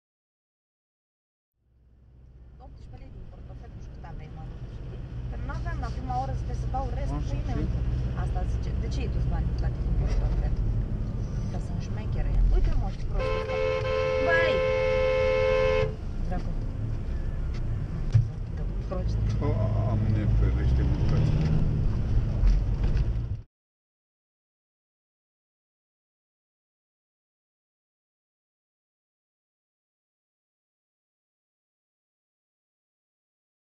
tupeu de hodorog
acesti soferi ai traficului comunist
precizez ca soferul, la fel ca pasagerul, era octogenar